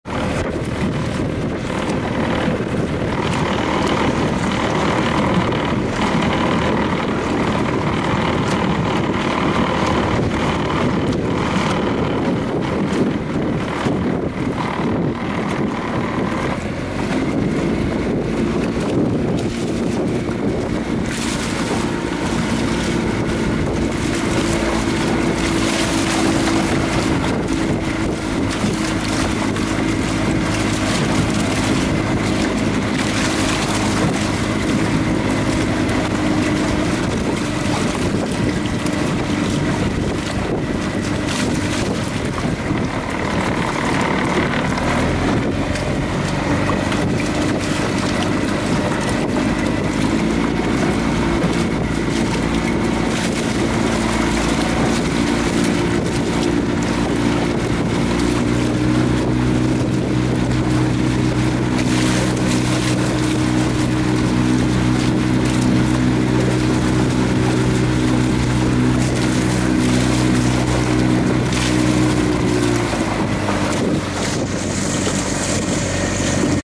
Trimaran hull in the chop
Eagle trimaran's port ama in a light chop and motoring along at 9knots